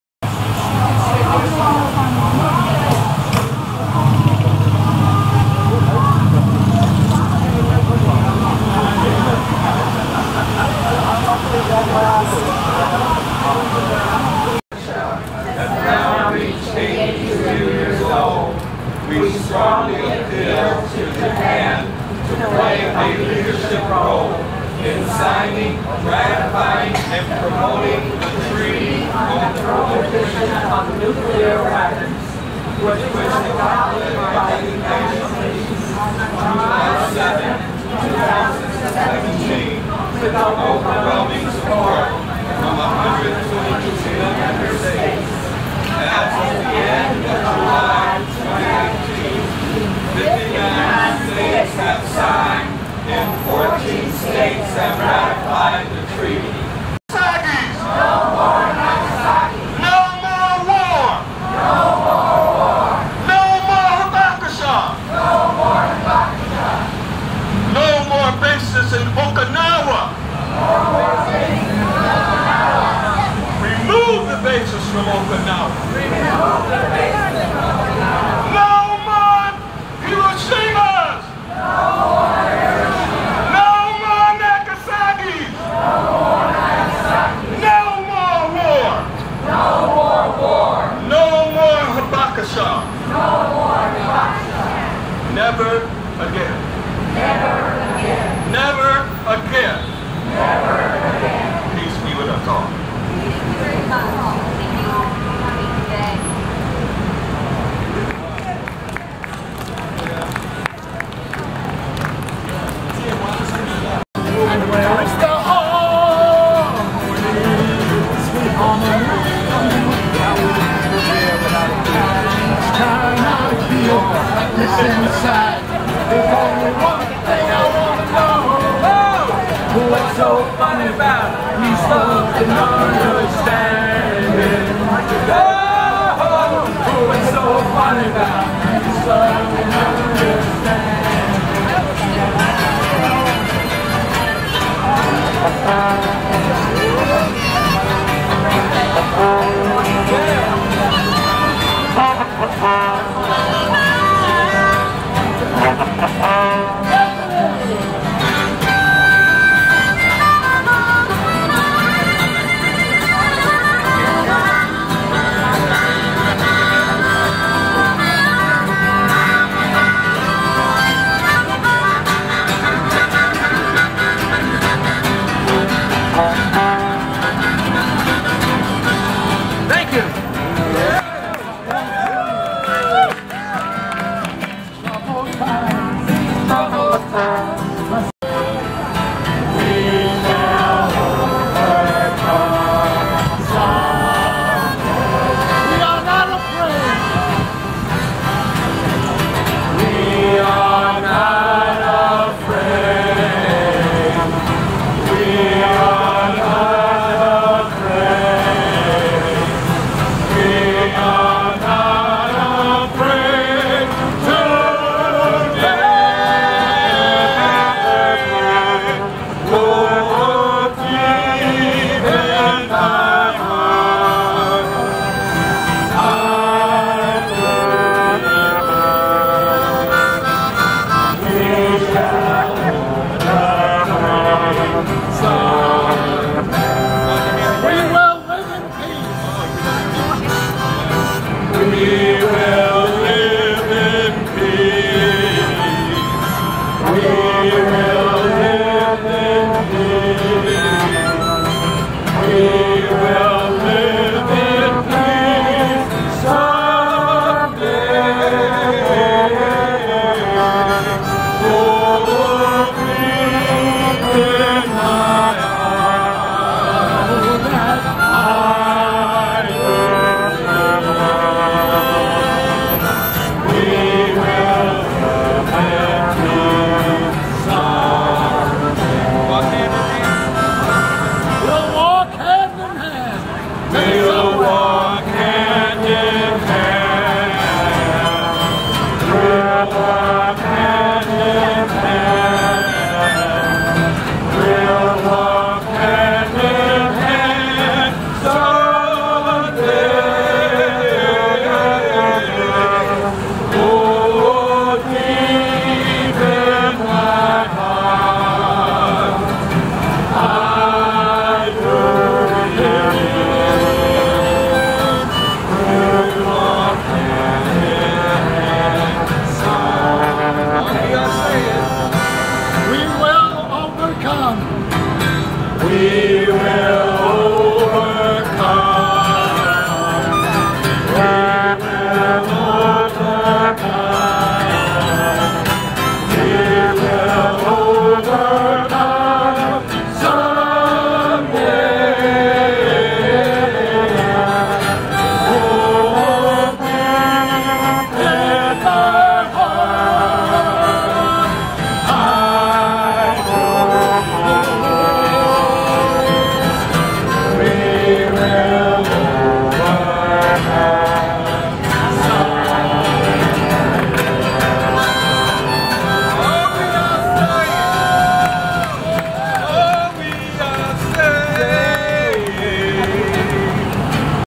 73rd Anniversary of Hiroshima and Nagasaki in NYC
Peace gathering to commemorate the 73rd anniversary of Hiroshima and Nagasaki at the Consulate general of Japan in New York City.